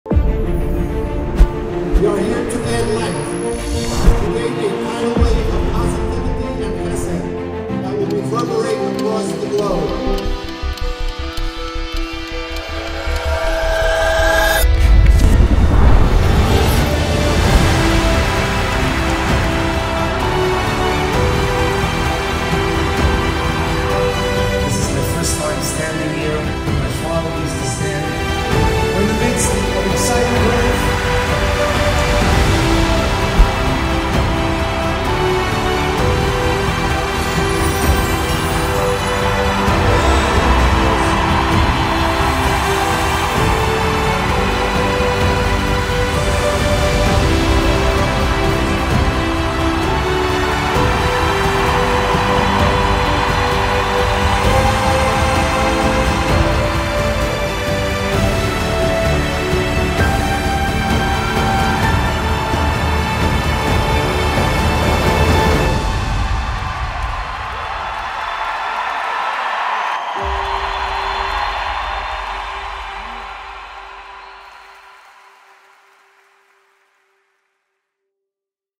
Highlights: HASC 37 at NJ Performing Arts Center
A Short Glimpse of HASC 37 - Sunday January 7 2024

the groundbreaking show that elevated Jewish music to thrilling new heights.

Among the performers were Avraham Fried, Shulem Lemmer, Benny Friedman, Mordechai Shapiro, Shmuel, DJ Matt Dubb, a Shlomo Carlebach Hologram, a beautiful tribute of Dedi songs with his son Shai Graucher, who flew in from Israel, a musical play with Abie Rottenberg performed by Chaim Brown and Leiby Weider.

Produced by Shlome Steinmetz of Pivot Productions.